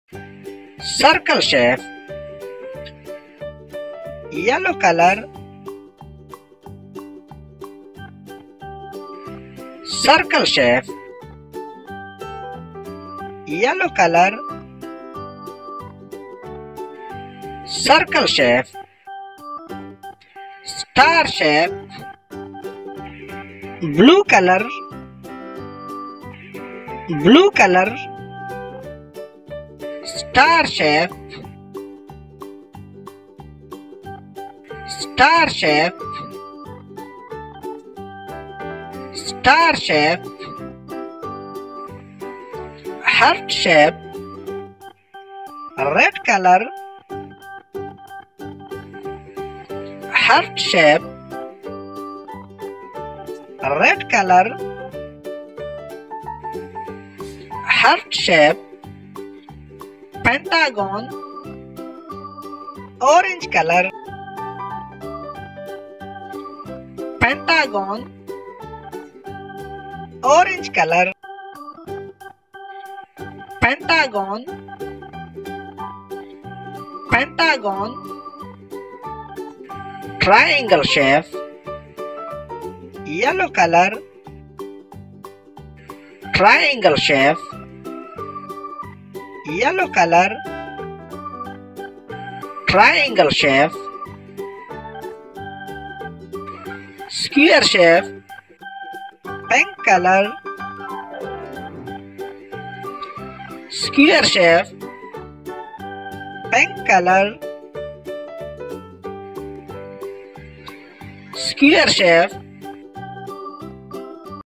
draw and color educational shape's | color painting drawing for kids
"Welcome to our fun and colorful learning video for kids! In this video, children will explore different shapes such as triangles, stars, hearts, and rectangles. Each shape is beautifully colored to engage young minds and make learning enjoyable. Perfect for preschoolers and toddlers, this educational video will help kids recognize and remember shapes in a fun way.
- Learning shapes
- Educational video for toddlers
- Preschool shapes
- Colorful shapes
- Shapes and colors
- Triangle, star, heart, rectangle